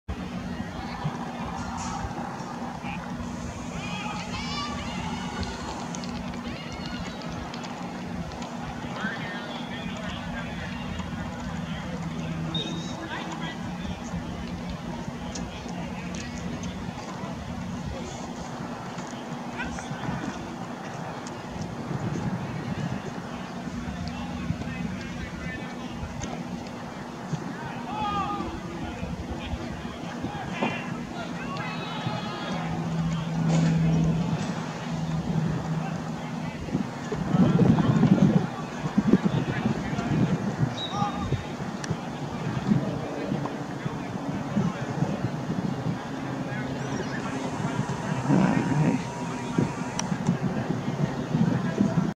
Unidentified Fast Moving Black Object (THUNDERBIRD??)
In 1948 a father and son witnessed a large bird (look up 'Thunderbird') that was black, flying at a high rate of speed at over 500 feet high. With it's wings back, it was described as looking like a Naval Torpedo. This object was captured in June 2018.